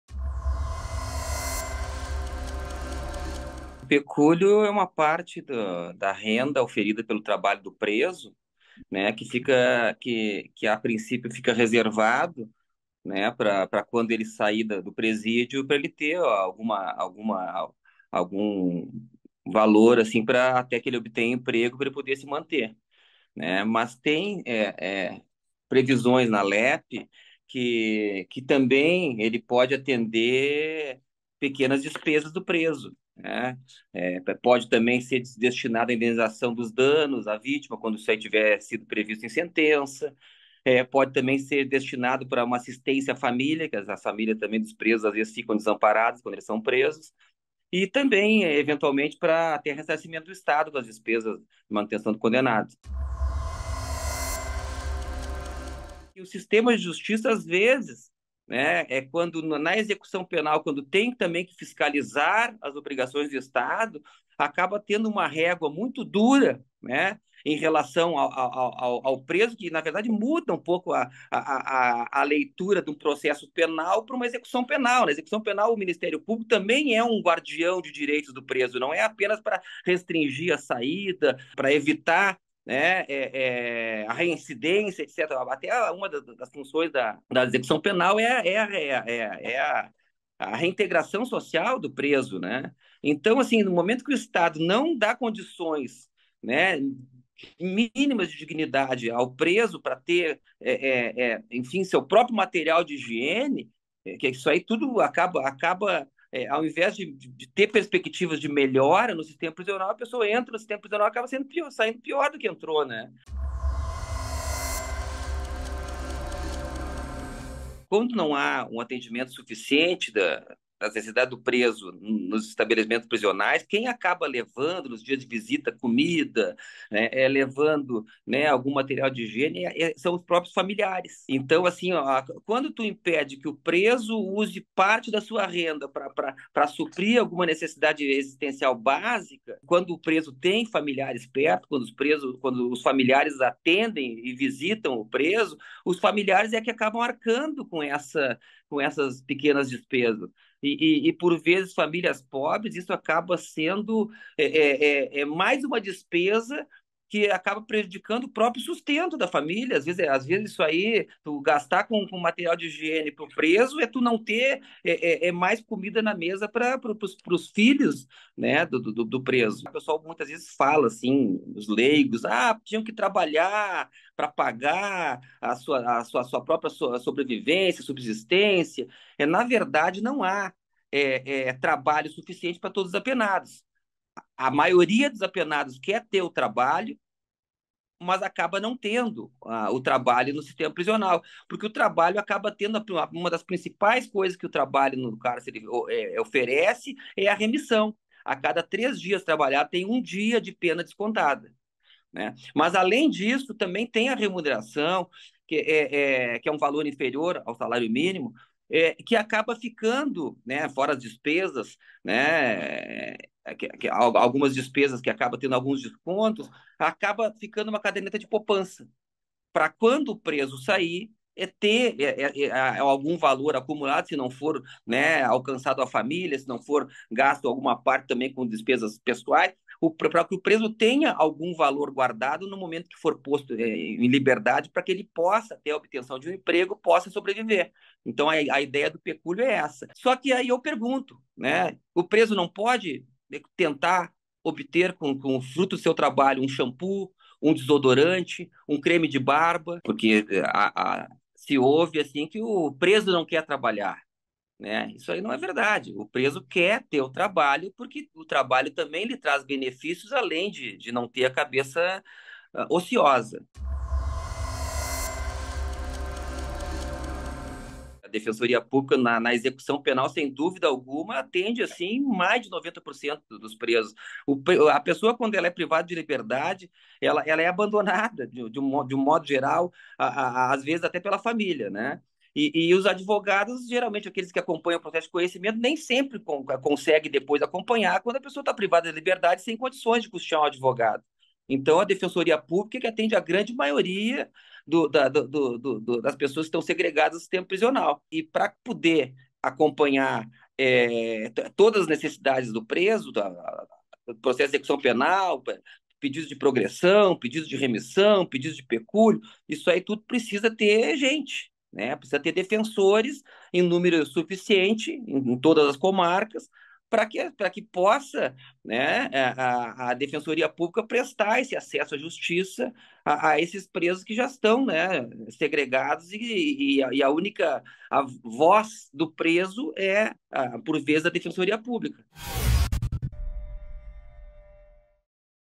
"Falta trabalho", diz defensor sobre pecúlio para item de higiene a preso
O defensor público Rafael Rapharelli, que acompanhou o REsp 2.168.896 no STJ, concedeu entrevista ao Migalhas destacando o papel essencial do pecúlio.

00:00 O que é pecúlio?
00:53 Lei de Execução Penal
01:53 Trabalho e remuneração
04:24 Defensoria Pública